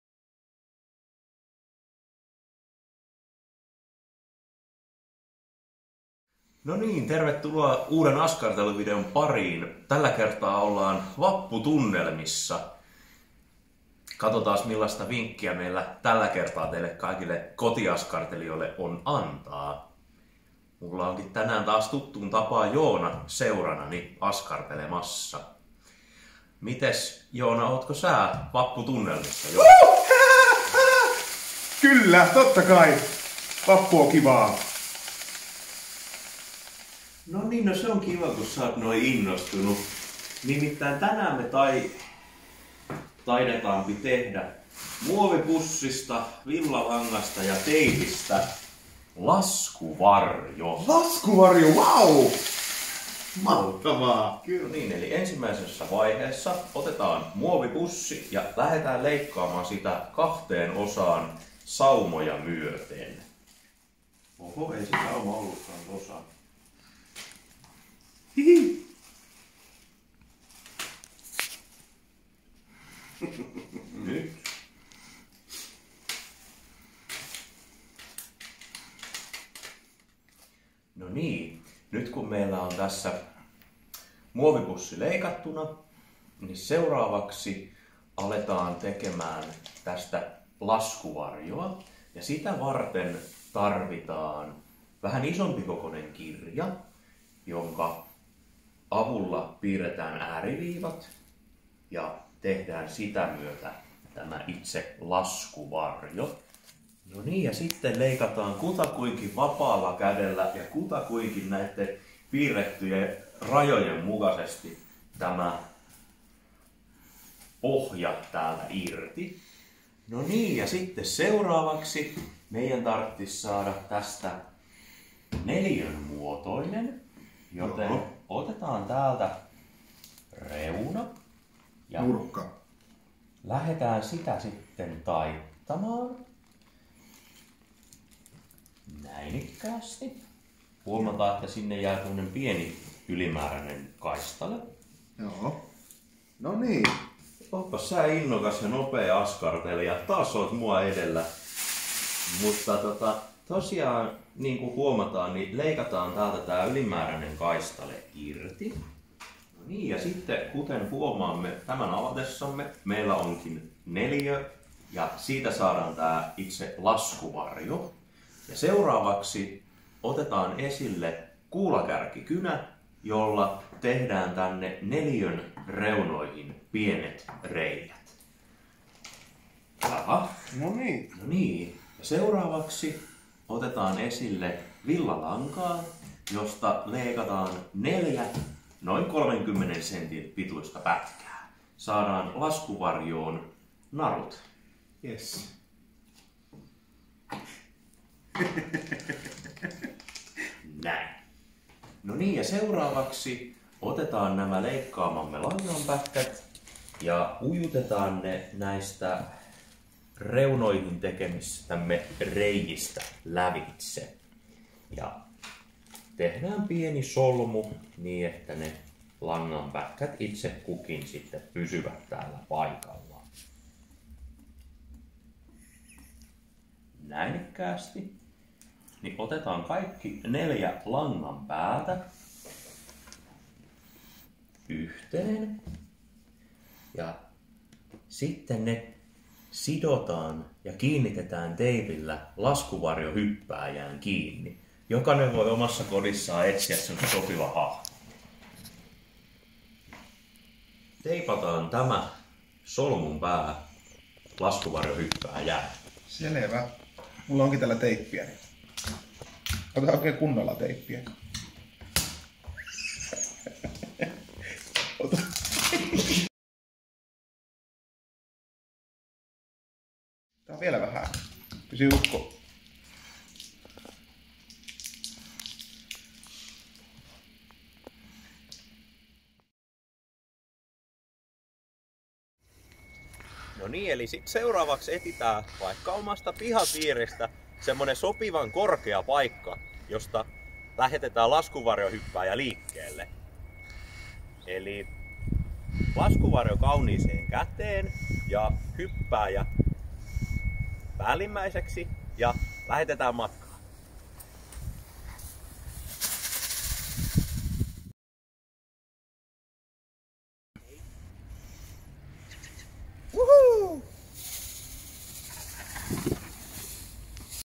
Askartele Oskarin ja Joonan kanssa, vappuspecial
Oskari ja Joona askartelevat vapuksi laskuvarjolelun.